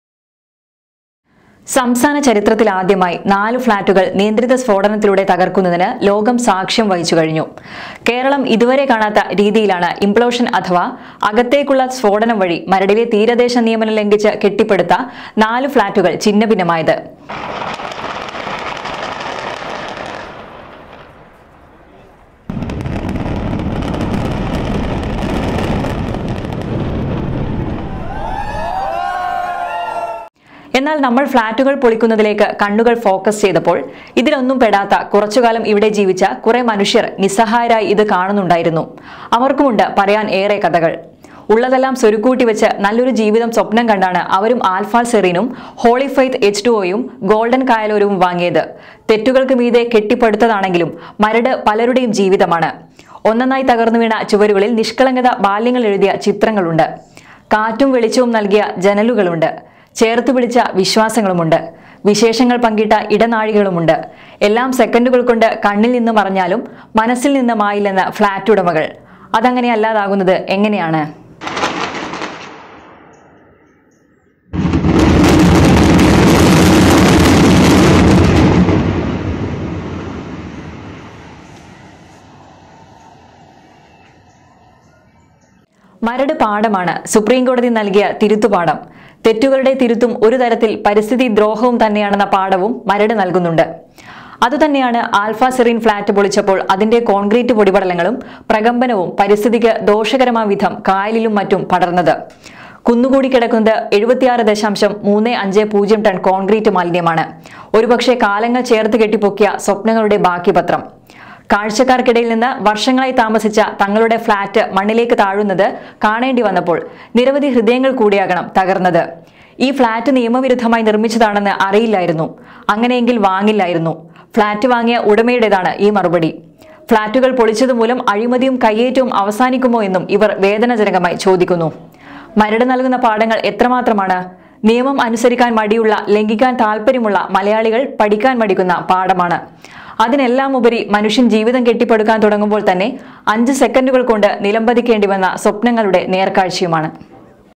മരട് | കാലങ്ങള്‍ ചേര്‍ത്ത് കെട്ടിപ്പൊക്കിയ സ്വപ്‌നങ്ങളുടെ ബാക്കിപത്രം
ഇവിടെയുണ്ടായിരുന്നു സ്വപ്നങ്ങൾ വാനോളം ഉയർത്തി ജീവിതം മുന്നോട്ടു കൊണ്ടുപോയ മനുഷ്യർ.. മരട് ഫ്ളാറ്റുകൾ തകർത്തപ്പോൾ ഛിന്നഭിന്നമായത് വാനോളം പ്രതീക്ഷകൾ